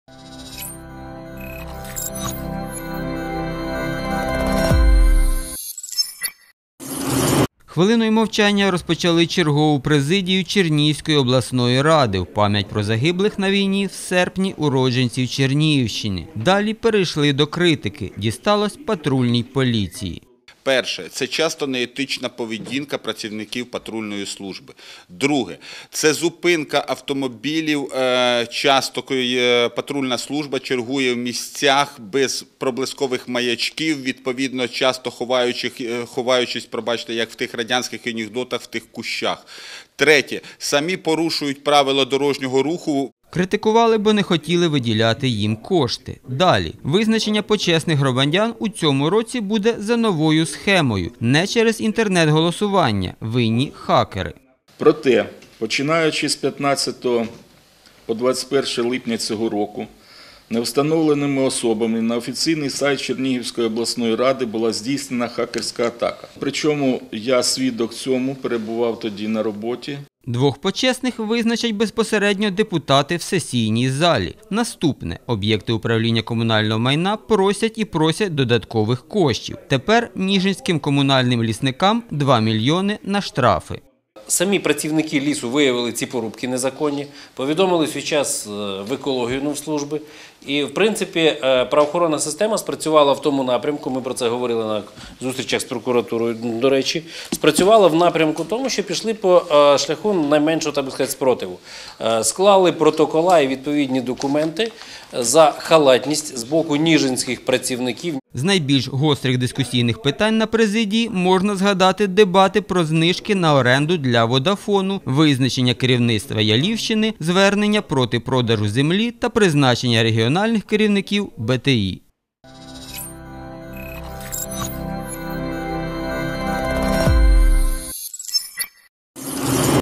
«Разнос» патрульной полиции и хакерская атака на почетных граждан
Президиум черниговского облсовета прошел в жарких дебатах депутатов. Некоторые из них особо раскритиковали патрульную полицию области.